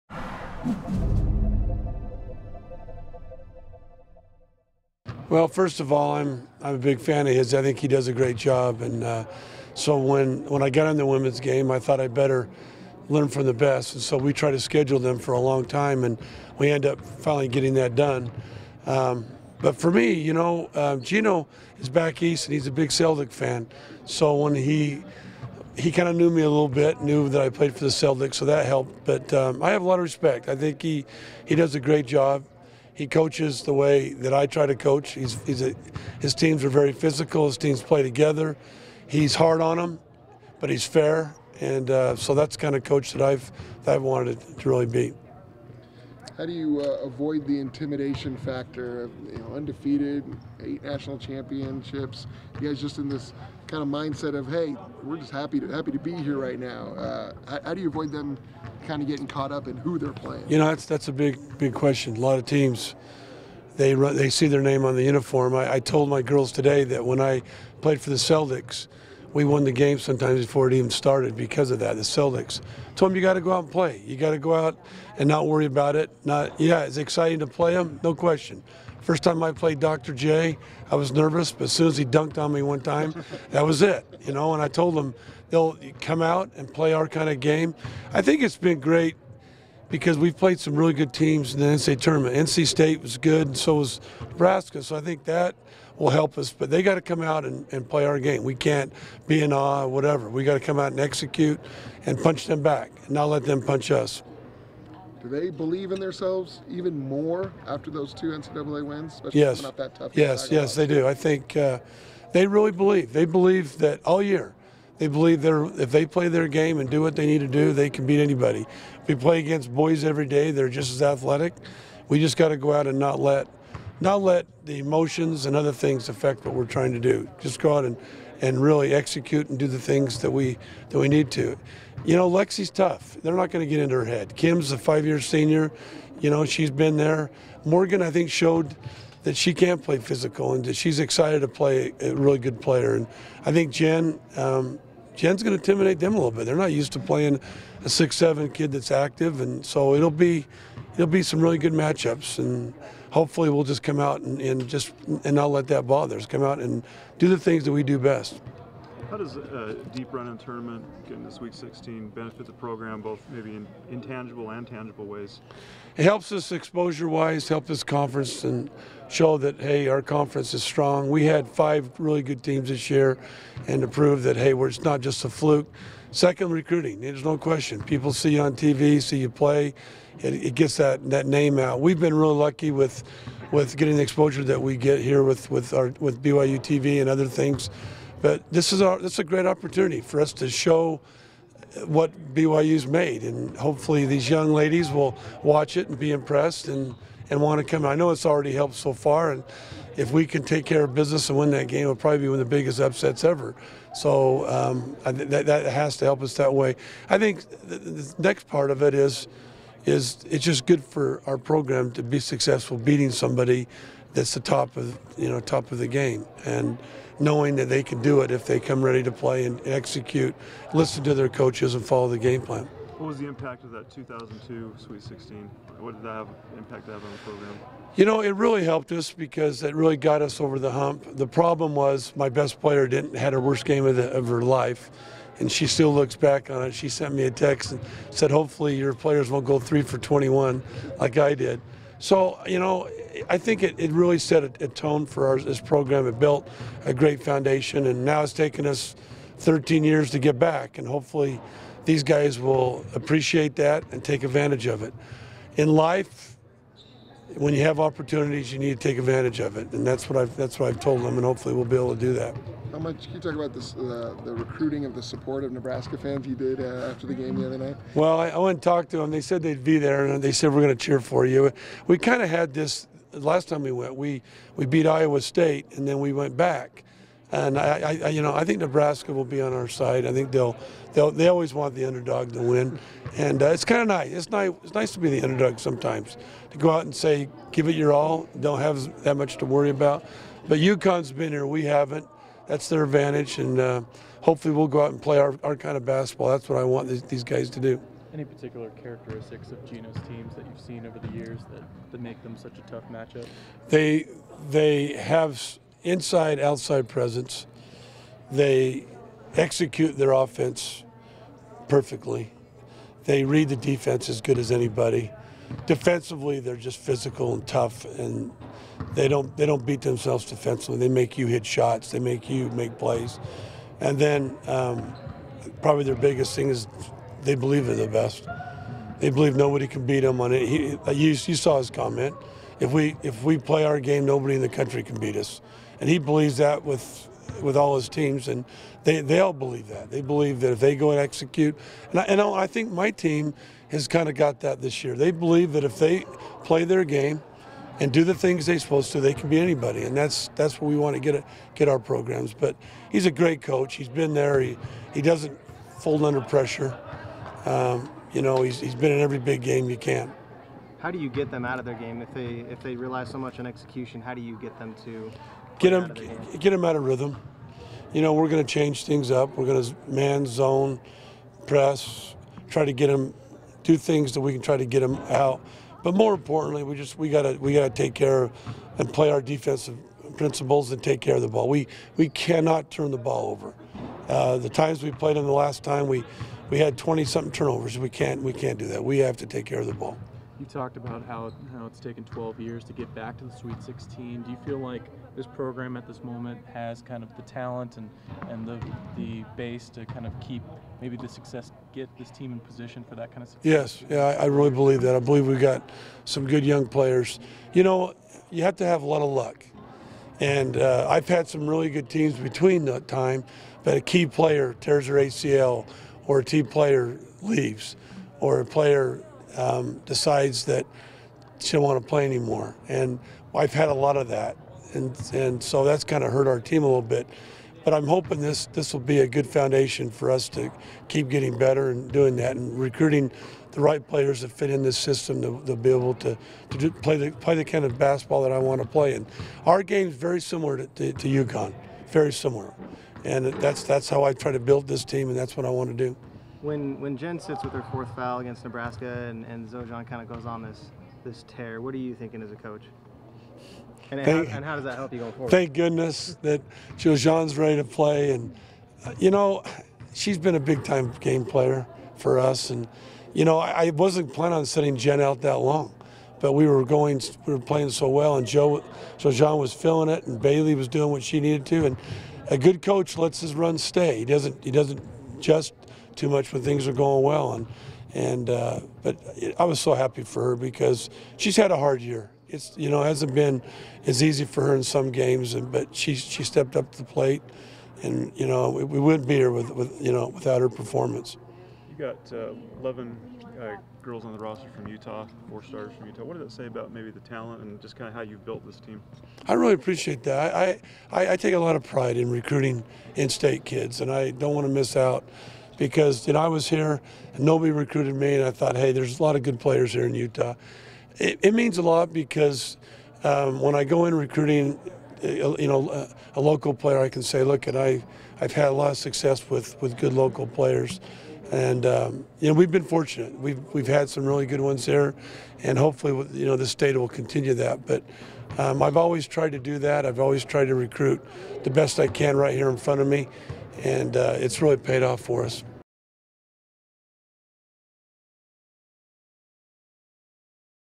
Jeff Judkins on his team's sweet 16 run
Jeff Judkins, BYU women's basketball head coach, talks about his team preparing to play against a historic UCONN team in the sweet 16.